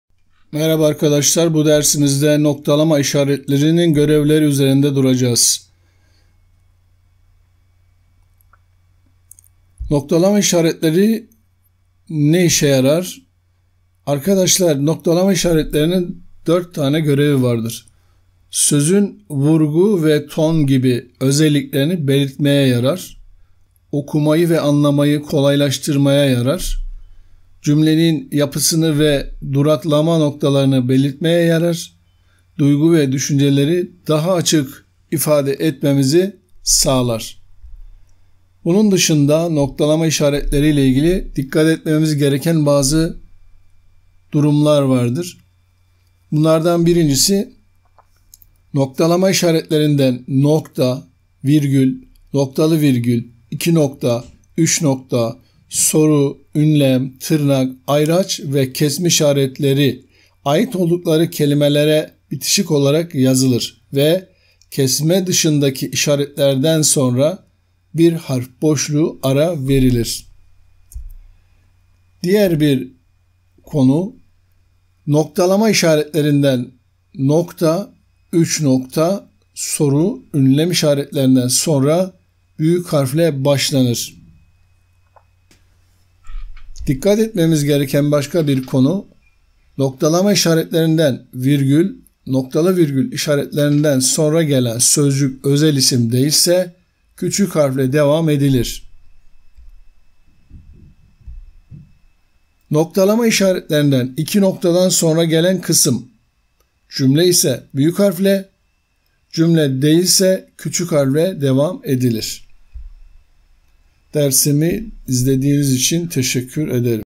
NOKTALAMA İŞARETLERİNİN GÖREVLERİ NELERDİR?
Noktalama işaretleri ne işe yarar? Noktalama işaretleriyle ilgili bilmemiz gereken önemli durumlar nelerdir?